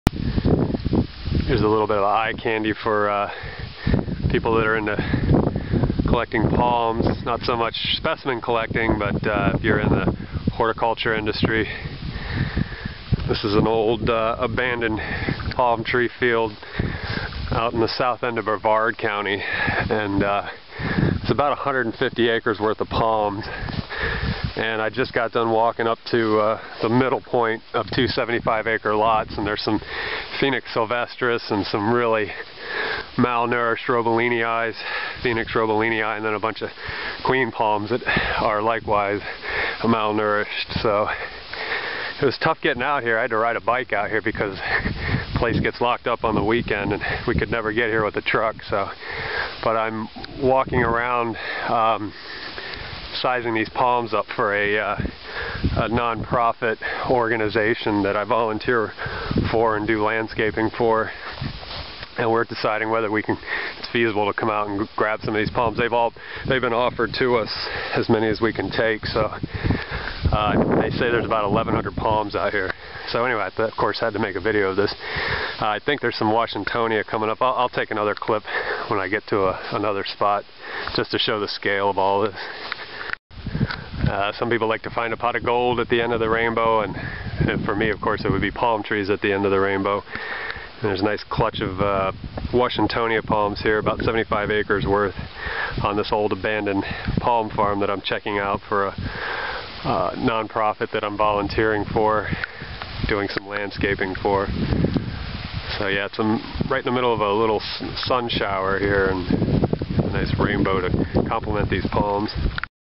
Huge Palm Grow in Central Florida
This is a video of a large palm grove that was abandoned. There are around 1100 palms on 150-200 acres. Nothing exotic but it's impressive to see so many palms in one place.